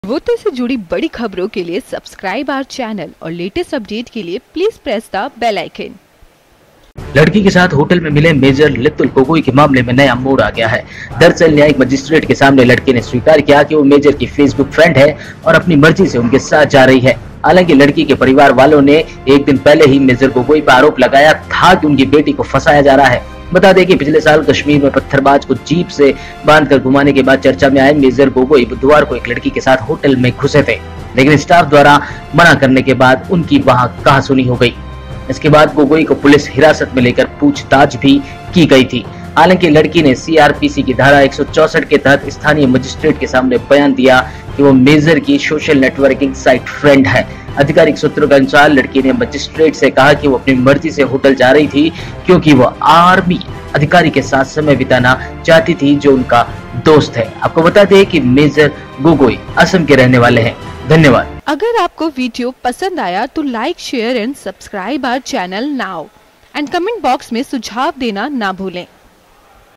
Major Gogoi के मामले में आया नया मोड़, लड़की ने कहाः मर्जी से गई थी
लड़की के साथ होटल में मिले मेजर लीतुल गोगोई के मामले में नया मोड़ आ गया है। दरअसल, न्यायिक मजिस्ट्रेट के सामने लड़की ने स्वीकार किया कि वह मेजर की फेसबुक फ्रेंड है और वह अपनी मर्जी से उनके साथ जा रही थी। हालांकि लड़की के परिवारवालों ने एक दिन पहले ही मेजर गोगोई पर आरोप लगाते हुए कहा था कि उनकी बेटी को फंसाया जा रहा है।